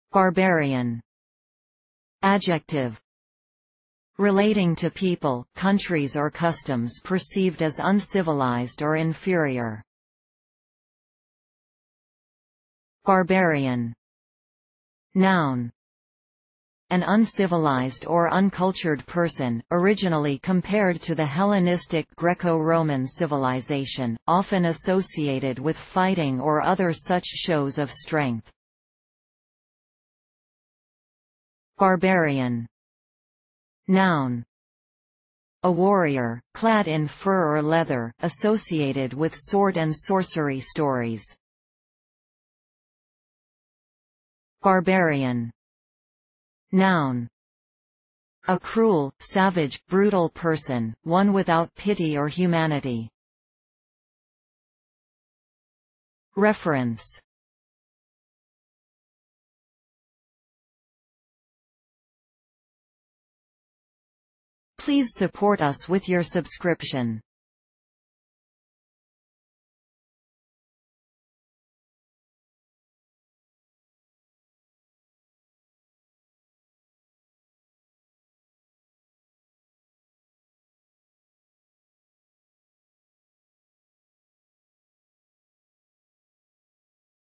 Barbarian | Definition of barbarian
See here, the definitions of the word barbarian, as video and text.

barbarian (adjective)
    Relating to people, countries or customs perceived as uncivilized or inferior.

barbarian (noun)
    An uncivilized or uncultured person, originally compared to the hellenistic Greco-Roman civilisation; often associated with fighting or other such shows of strength.

barbarian (noun)
    A warrior, clad in fur or leather, associated with sword and sorcery stories.

barbarian (noun)
    A cruel, savage, brutal person; one without pity or humanity.

Reference:
    barbarian